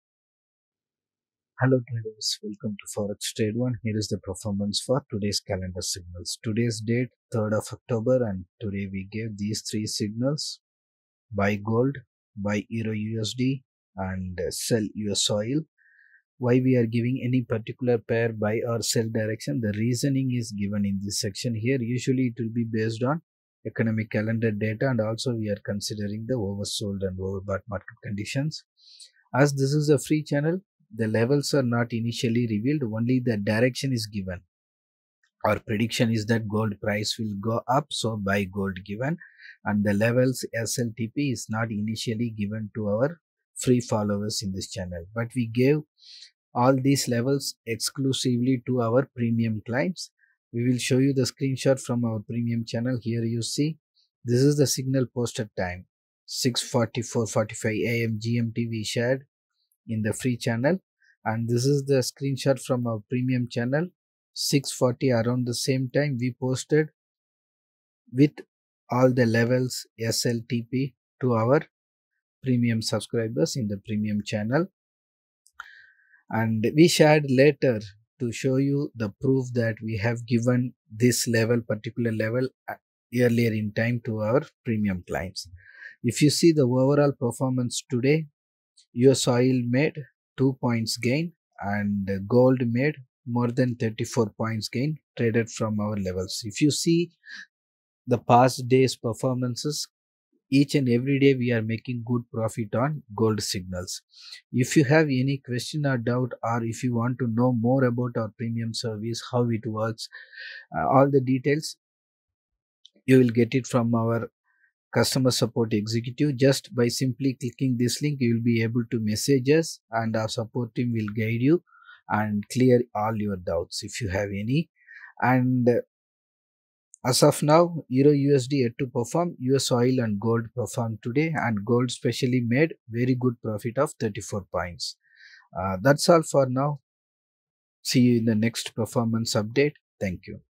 03 10 2024 CALENDAR SIGNAL PERFORMANCE
3rd October Calendar Signal Report

» BUY XAUUSD » 46 Point Gain=1lot= $4600

» SELL USOIL » 2.1 Point Loss=10lot= $2100

» BUY  EURUSD »  30 Pips Gain=2lot= $600

= Total = $7300

»10k Capital = 73.00%ROI=High Risk
»100k Capital= 7.3%ROI=Medium Risk
»1M capital= 0.73%ROI=Low Risk